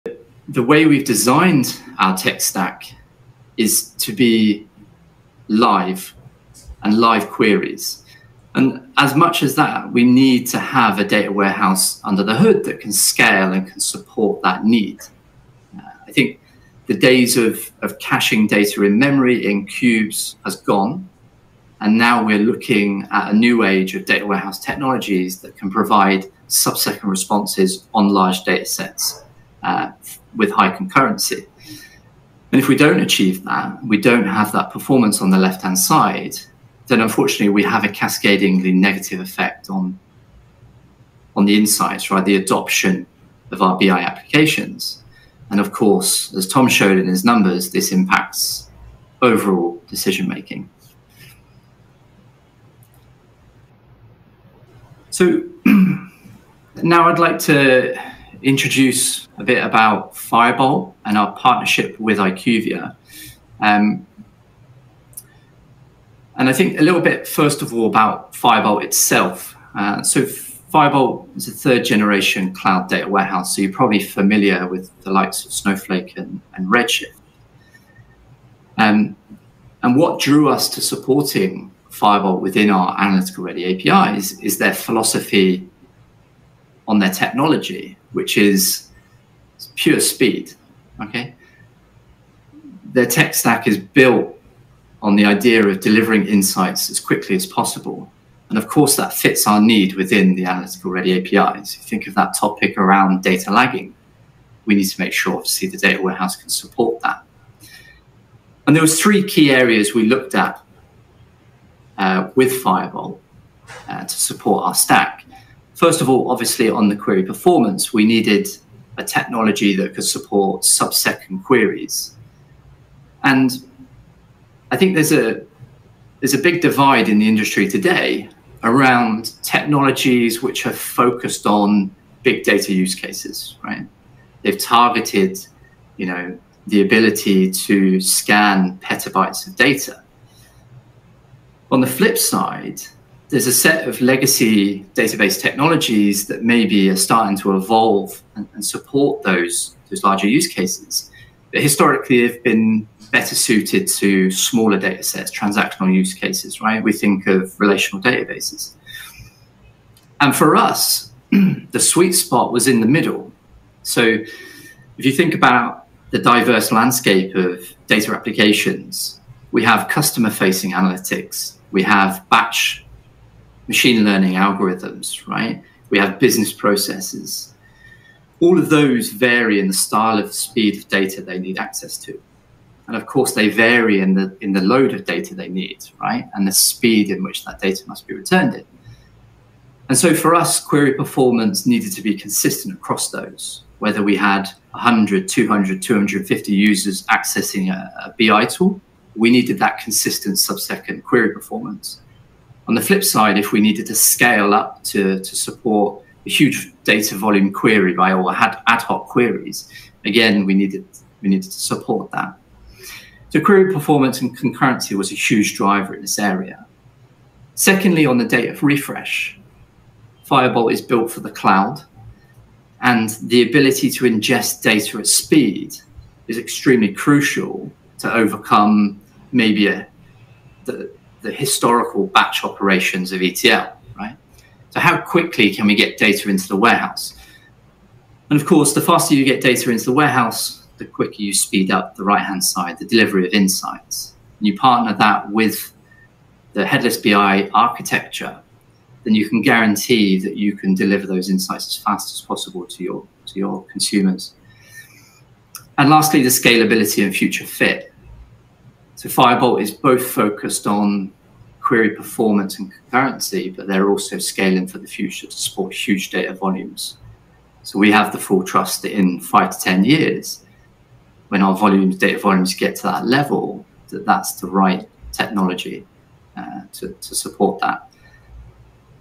Why IQVIA chose Firebolt for Life Science analytics
"Whether we have 100, 200, or 250 users accessing a BI tool, we need consistent sub-second query performance. Firebolt is a key partner for us." Jeremy Stroud, Director IT Architecture at IQVIA shares how IQVIA uses Firebolt to accelerate Life Science analytics.